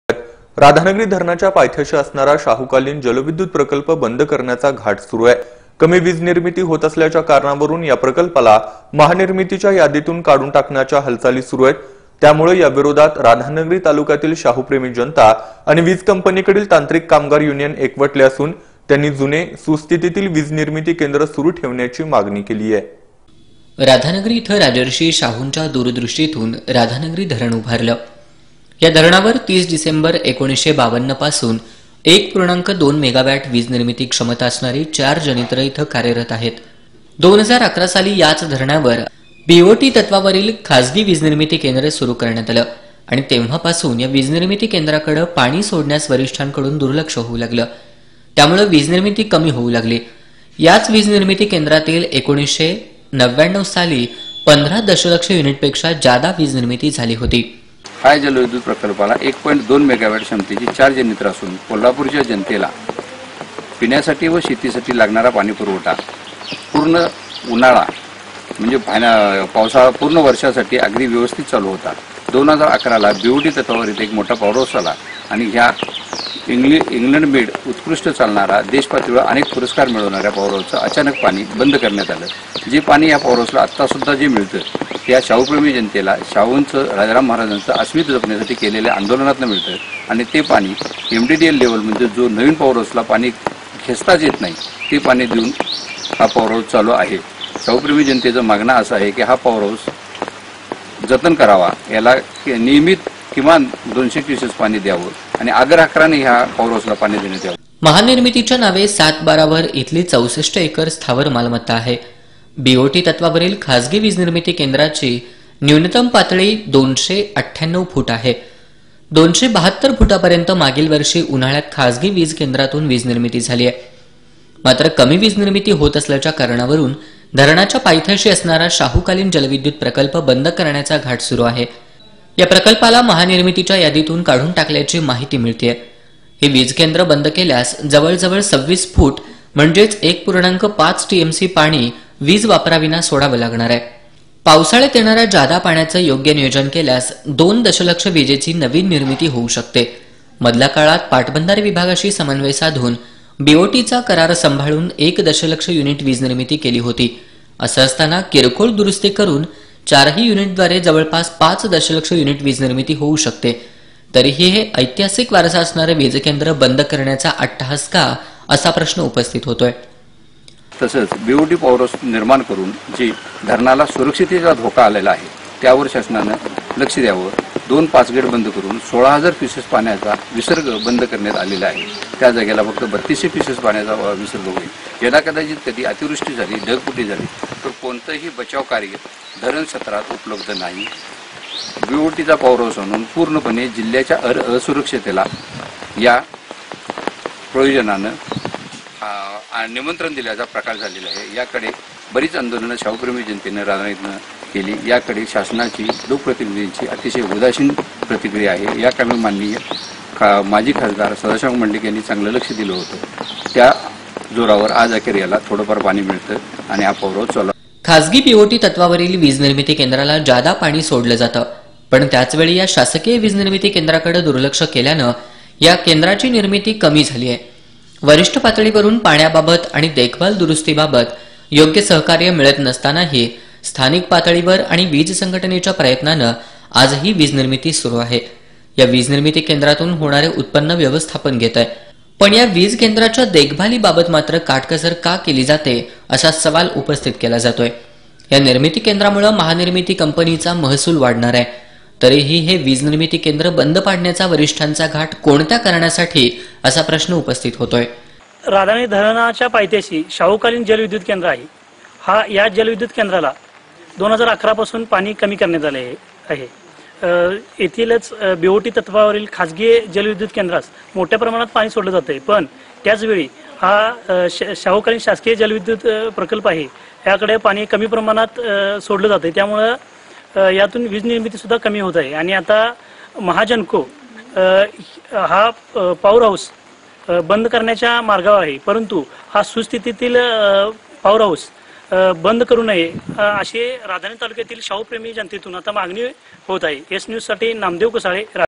S news kolhapur
राधानगरी धरण स्थळावरील जल विद्युत केंद्र बंद करण्याचा घाट